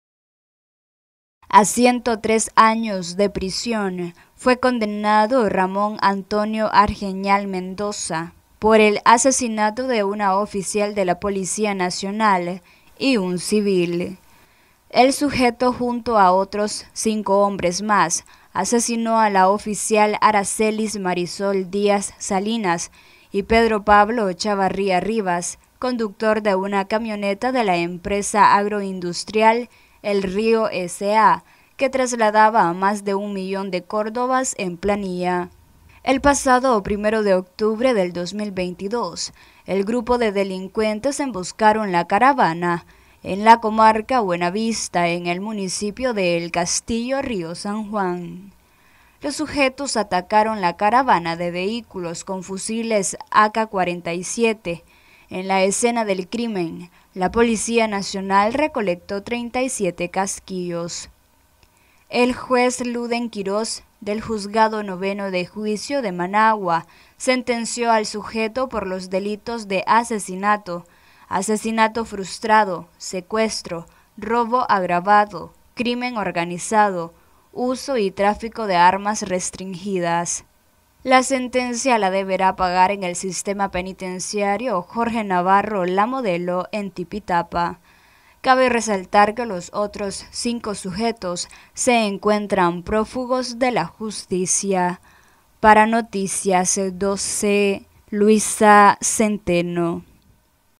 Noticias12Nicaragua Condenan a 103 años de prisión a sujeto que asesinó a oficial de la PN
Condenan a 103 años de prisión a sujeto que asesinó a oficial de la PN